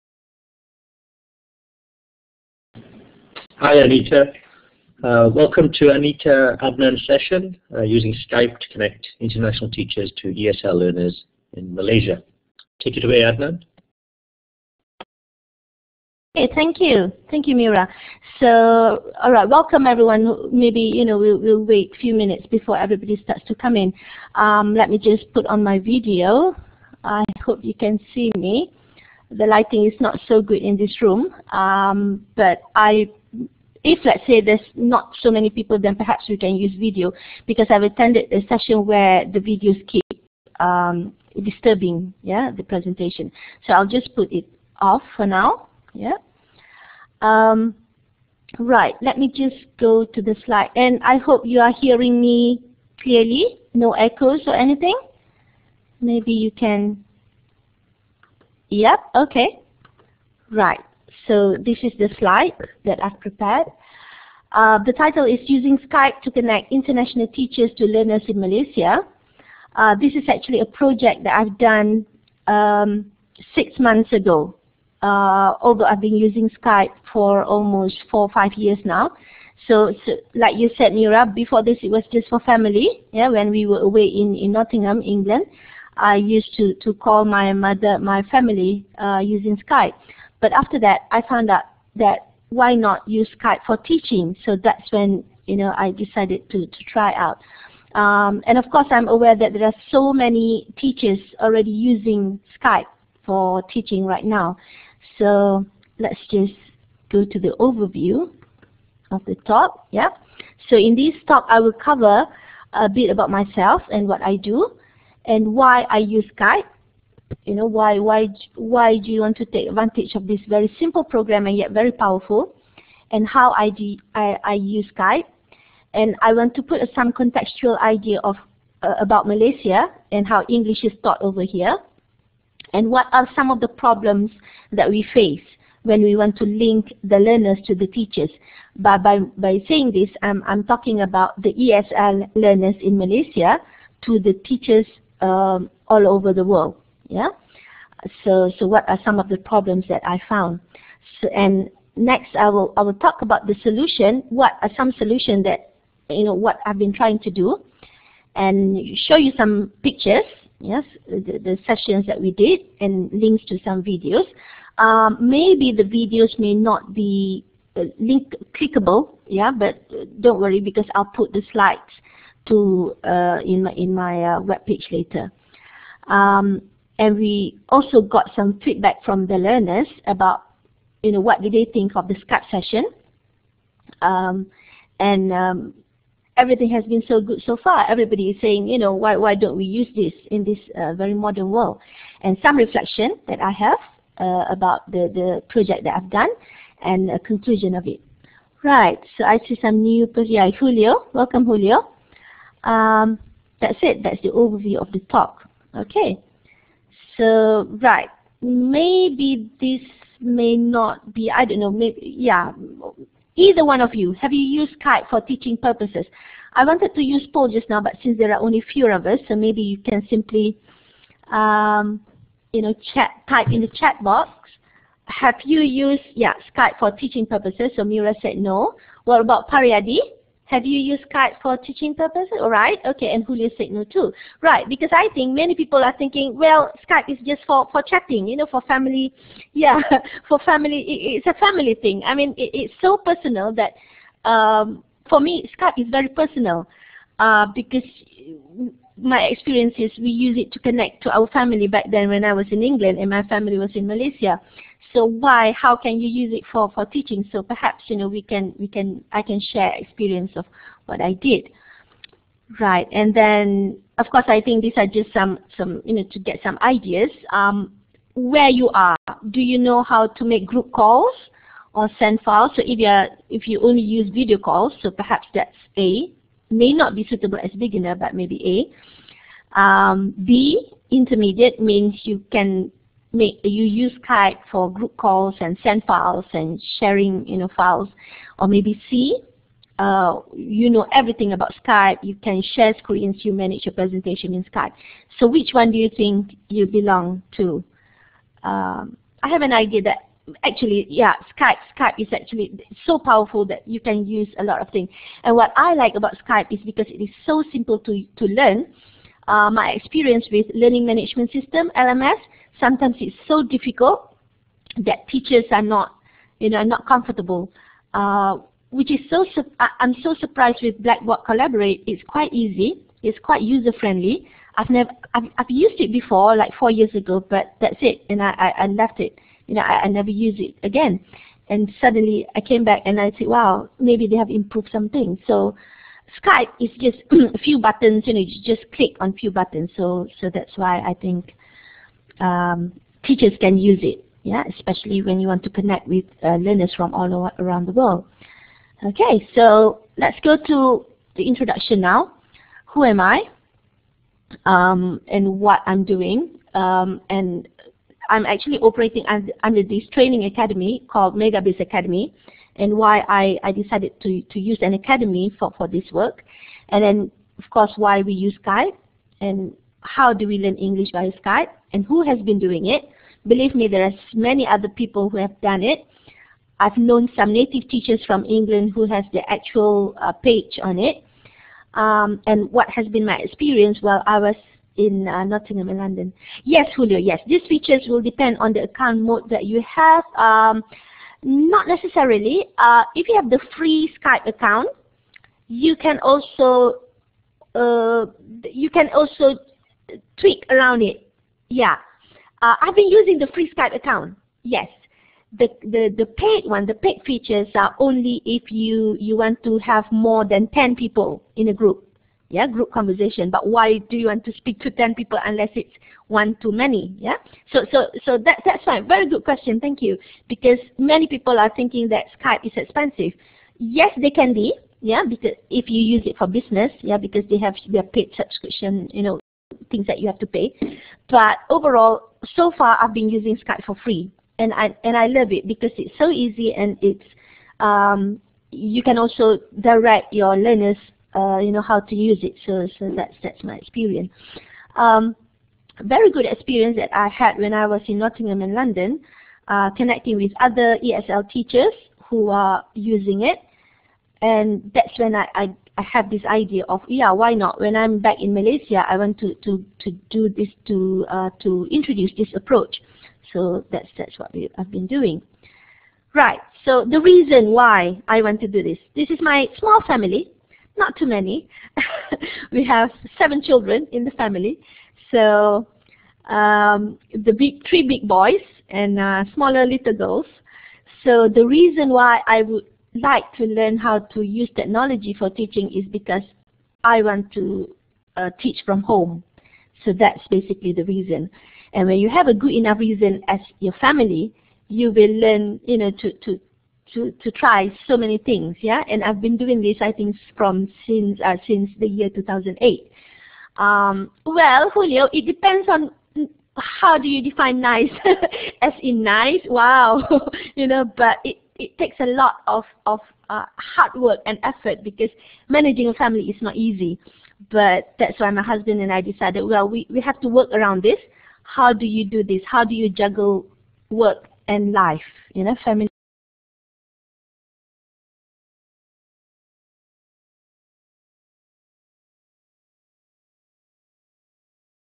Using Skype to connect international teachers to ESL learners in Malaysia by Anita Adnan
Reform Symposium Online Conference 11-13 October 2013

My presentation in RSCON4